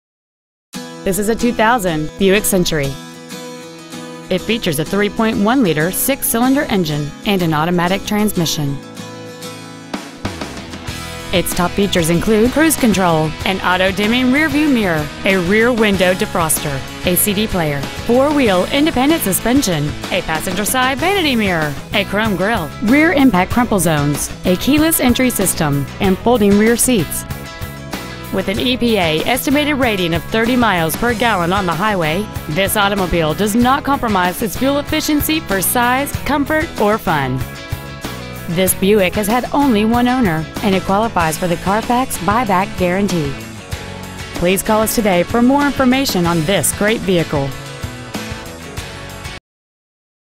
Buick Century Tulsa OK
We are proud to present this 2000 Buick Century.

 Year: 2000
 Make: Buick
 Model: Century
 Engine: V6 3.1 Liter
 Trans.: Automatic
 Exterior: White
 Miles: 94,293
 Interior: Gray

 6868 East B.A. Frontage Rd.

 Preowned 2000 Buick Century Tulsa OK